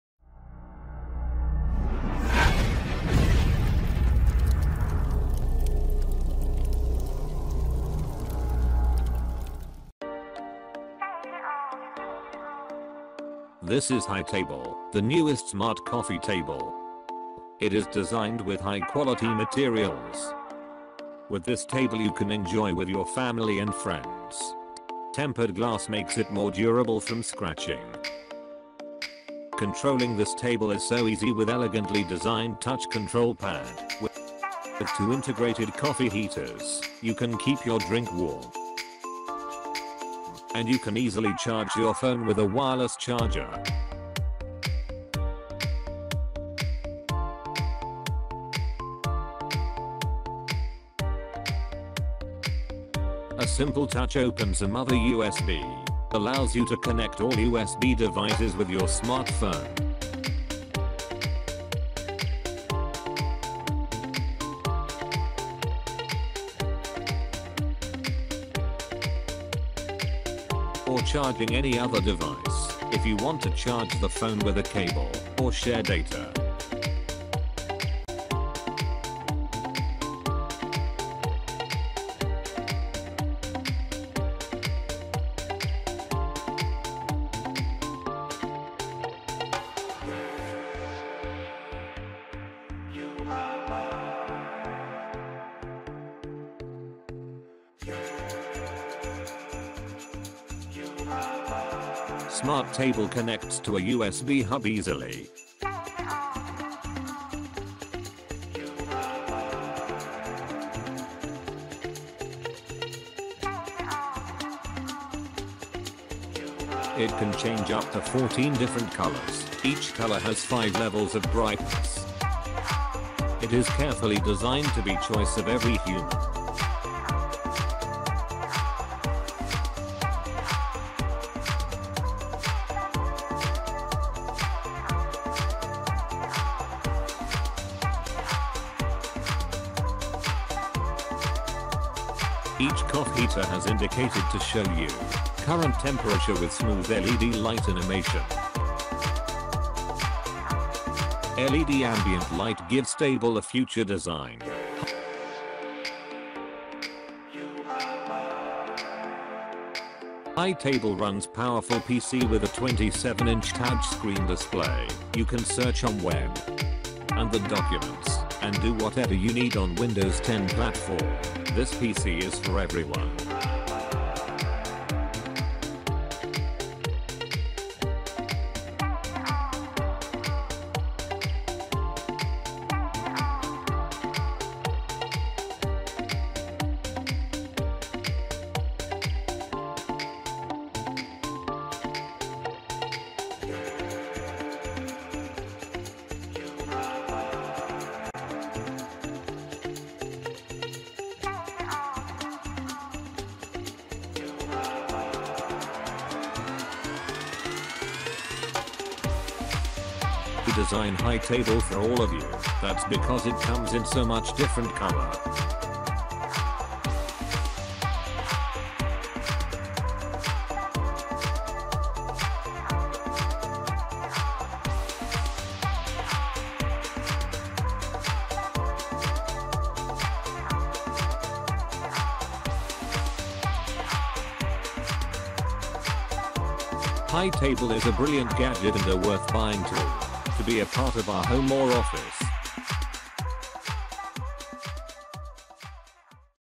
HITABLE - Smart Coffee Table For Your Home and Office
Smart coffee table with powerful PC, 27 inches touch screen display which is used as a proper computer system, Bluetooth speakers, wireless charger, Two coffee heaters, ambient LED light, digital clock, tempered glass, high quality materials, two child-safety power outlets, automatized USB hub.

All in one , that is only Hitable the smart coffee table for your home and office